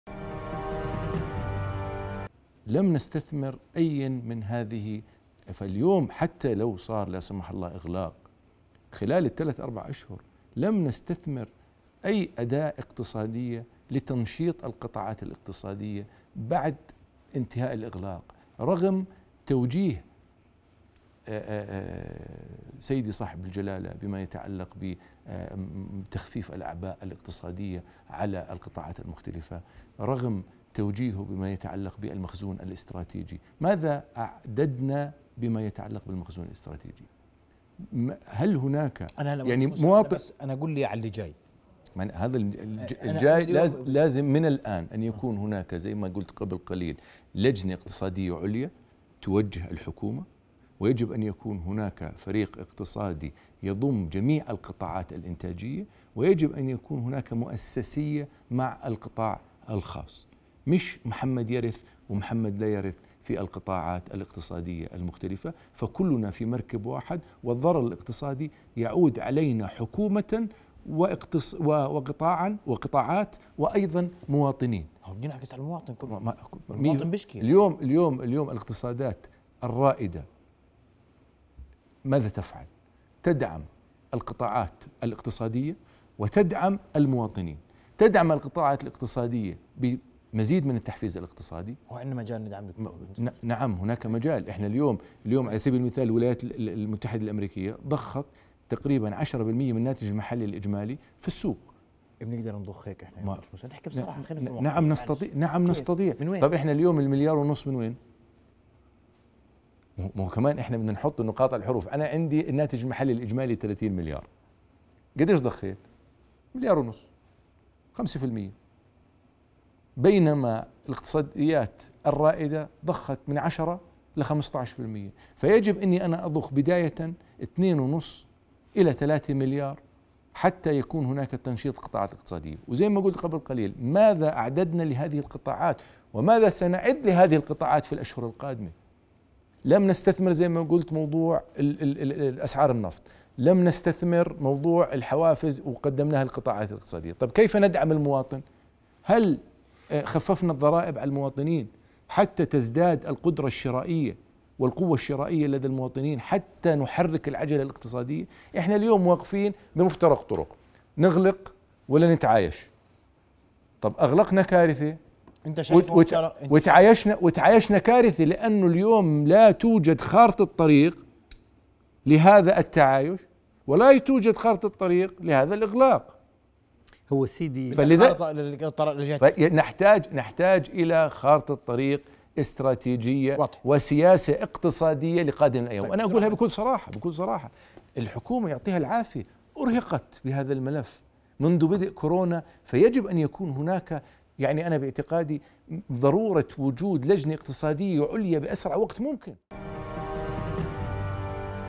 م. موسى الساكت: لا توجد خارطة طريق للتعايش مع كورونا ولا مع الإغلاق | 13-09-2020
م. موسى الساكت: لا توجد خارطة طريق للتعايش مع كورونا ولا مع الإغلاق